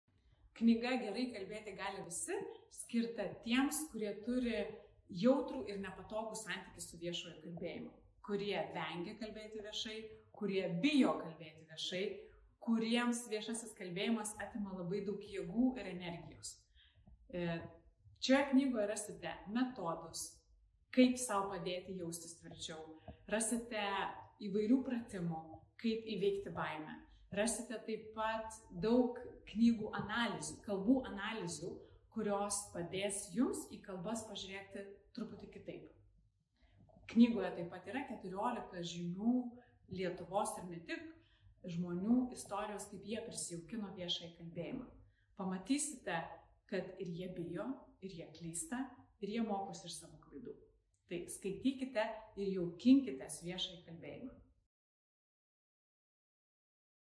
KALBA AUTORIAI. NERINGA BLIŪDŽIŪTĖ | Alma littera
Esate iš tų žmonių, kuriems viešasis kalbėjimas yra viena nepatogiausių situacijų, atimanti daug jėgų ir sukelianti nemalonias emocijas? Viešojo kalbėjimo ekspertė Neringa Bliūdžiūtė pristato naujausią knygą „Gerai kalbėti gali visi“ ir kviečia prisijaukinti viešąjį kalbėjimą!
Patarimų, naudingų metodikų, žymių žmonių kalbų analizių bei istorijų pripildyta knyga tiks visiems – ir manantiems, jog viešasis kalbėjimas – ne jiems, ir privalantiems kalbėti, bet nejaučiantiems tam meilės, ir net tiems, kurie kalbėdami prieš auditoriją dažniausiai jaučiasi patogiai.

Susipažinę su tinkama viešojo kalbėjimo technika įgyvendinsite tai, kas iki šiol atrodė neįmanoma!